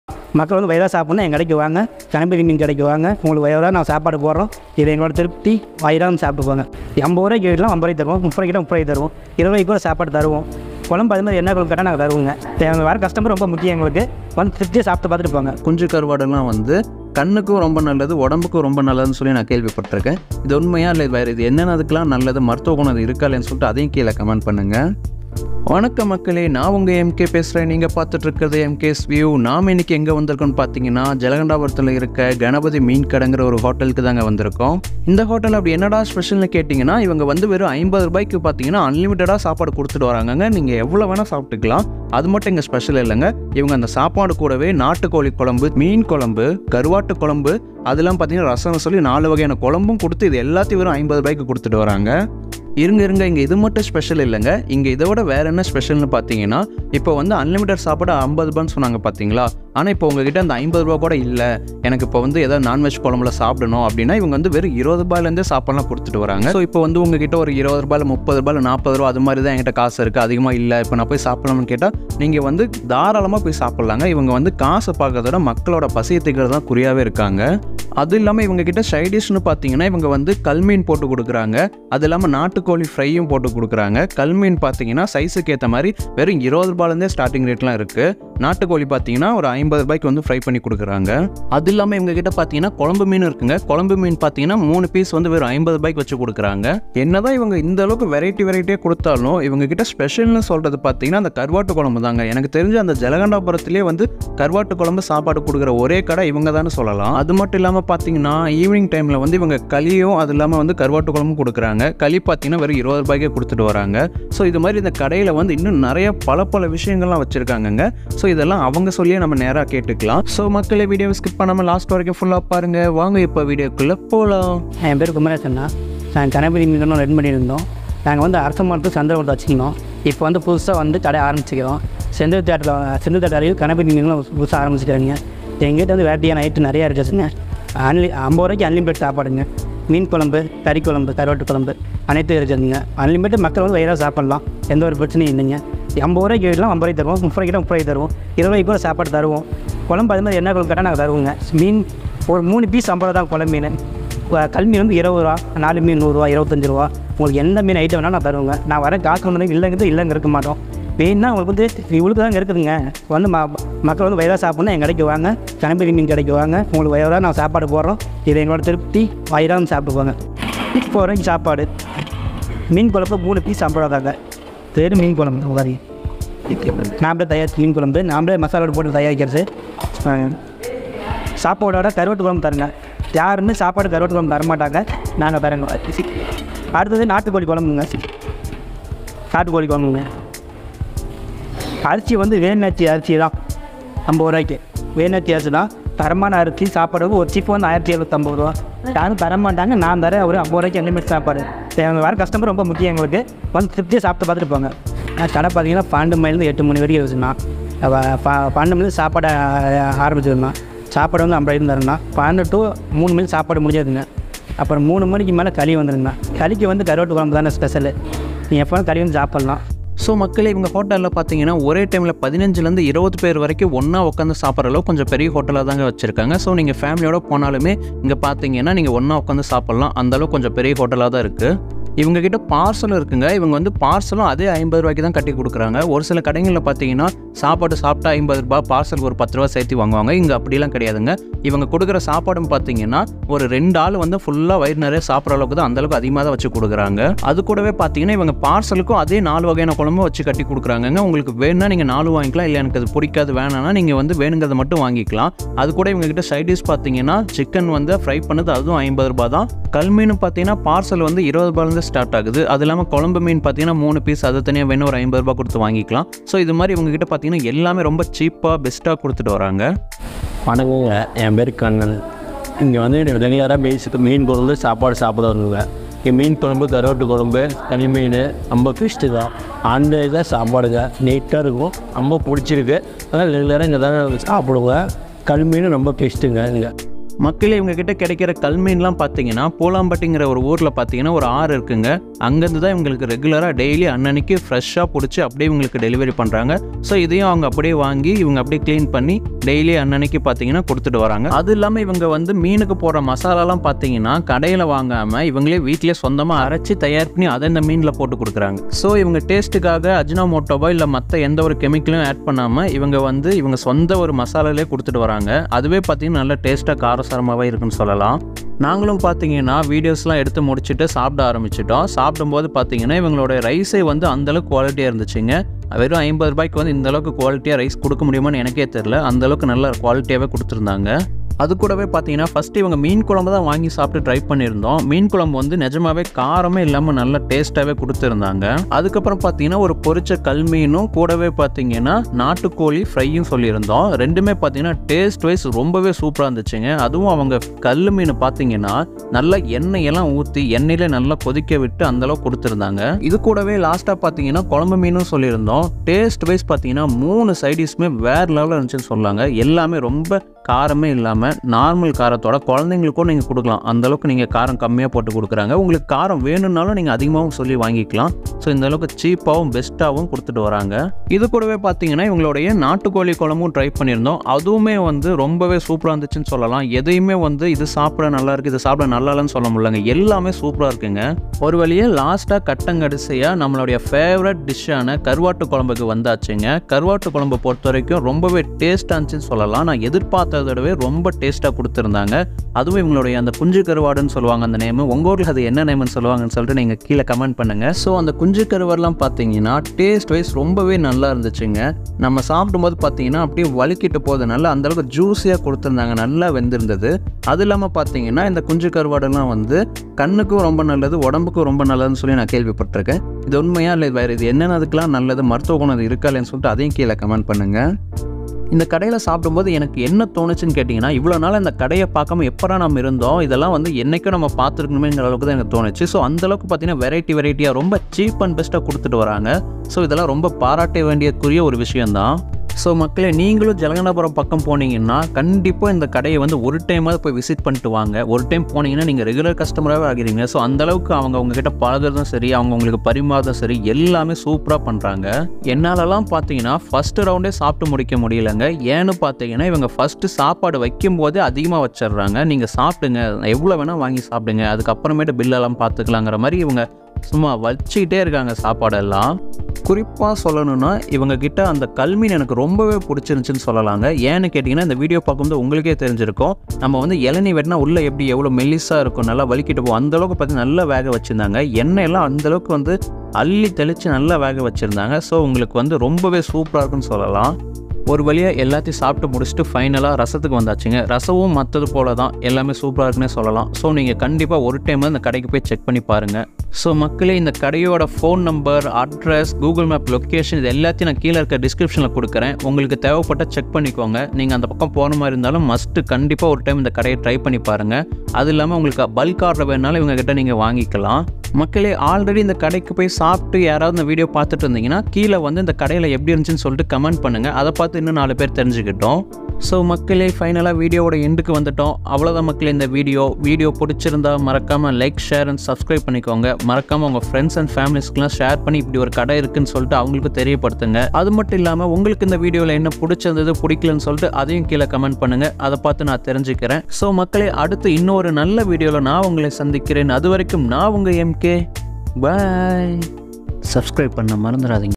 🛑 20₹ ரூபா சேலம் கல்மீன் | கருவாட்டு சோறு 4 வகை கறி குழம்புடன் கணபதி மீன் கடை - MKs View
😱 கருவாடு மற்றும் அளவில்லா சாப்பாடு 4 வகை கறிகுழம்புடன் ரூ 20 முதல் | fishfry dryfish - MKs View

Hotel Name (ஹோட்டல்) - கணபதி மீன் கடை

📍கணபதி மீன் கடை Adress -
Salem Dt, jalagandapuram, Nearby senthil theatre (சேலம் மாவட்டம், ஜலகண்டபுரம், செந்தில் தியேட்டர் அருகில் )